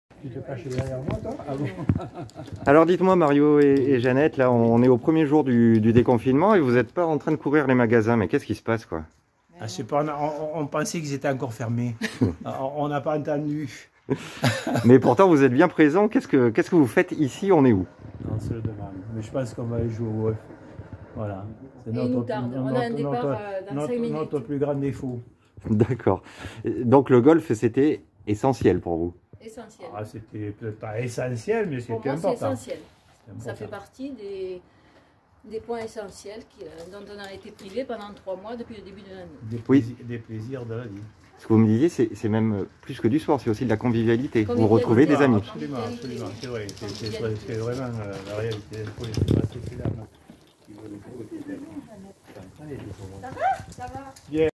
lot et garonne : les sportifs amateurs se ruent les terrains
au golf du château d'Allot les golfeurs se sont rués sur le parcours dès le samedi du deconfinement